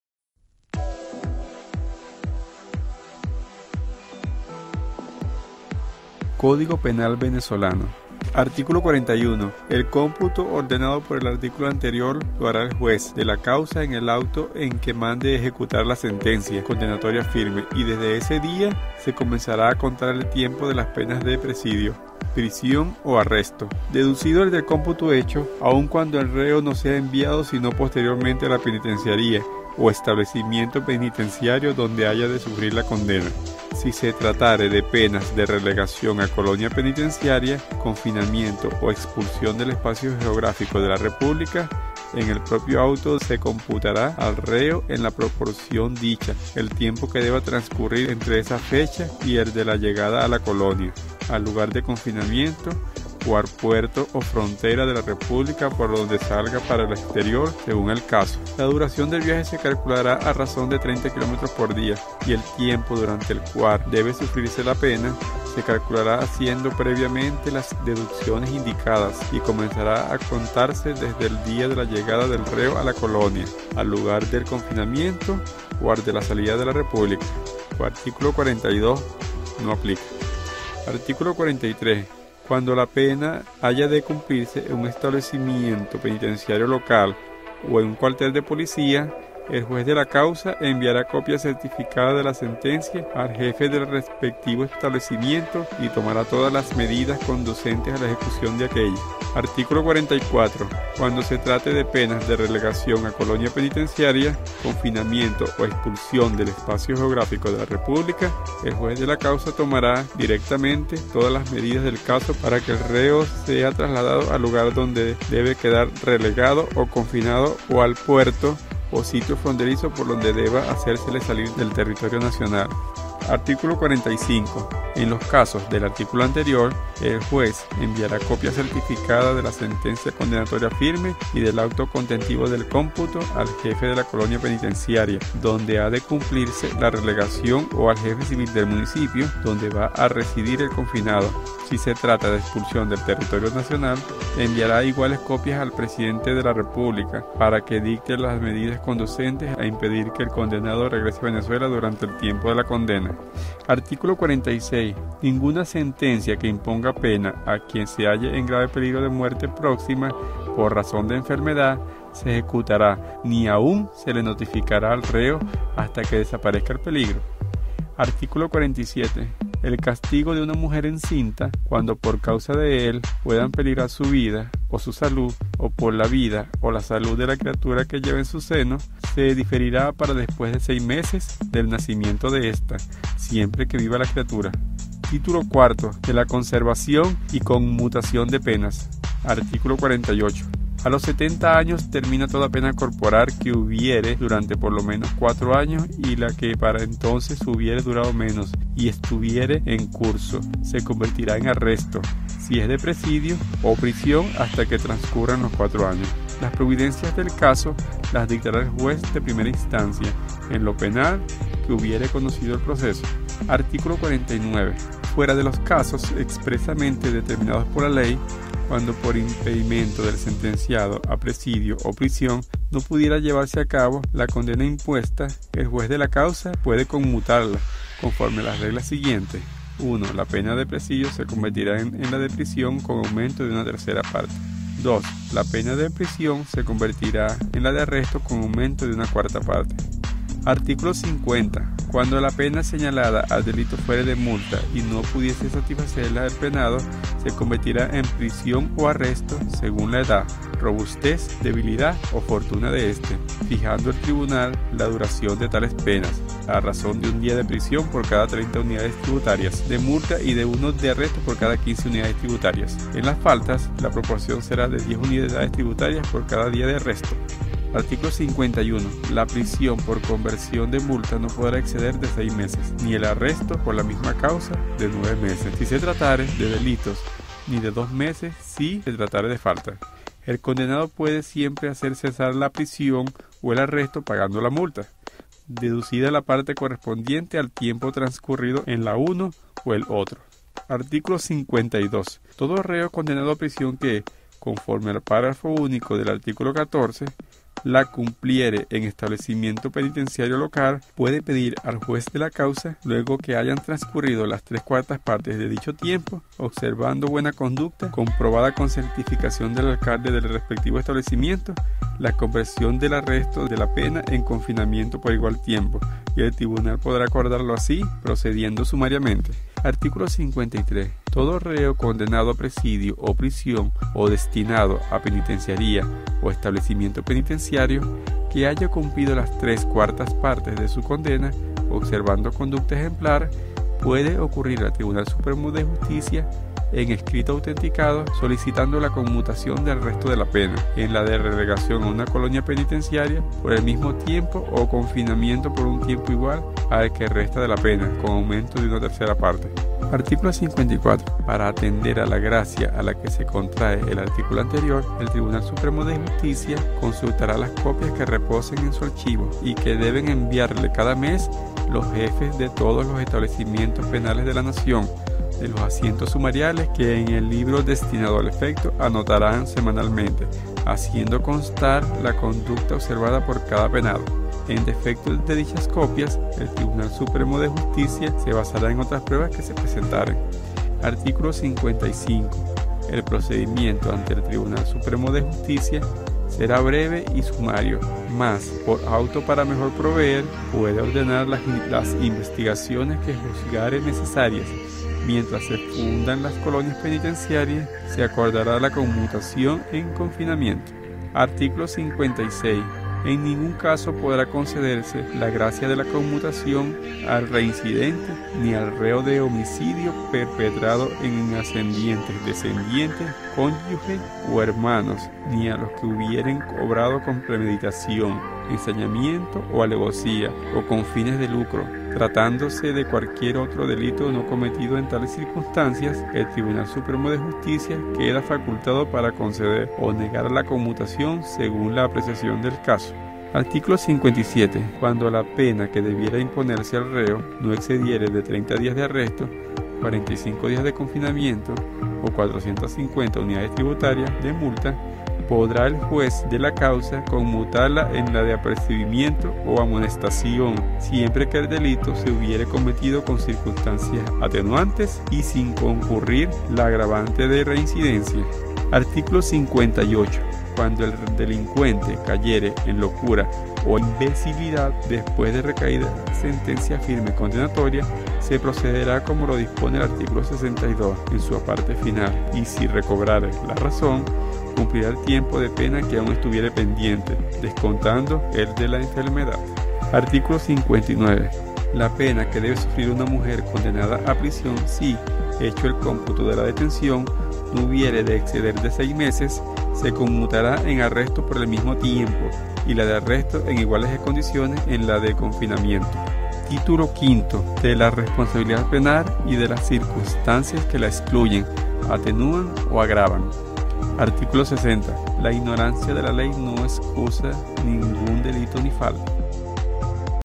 Codigo Penal Venezolano, Art. 41 al 60 (audio)
Un aporte para todas aquellas personas con dificultades de lectura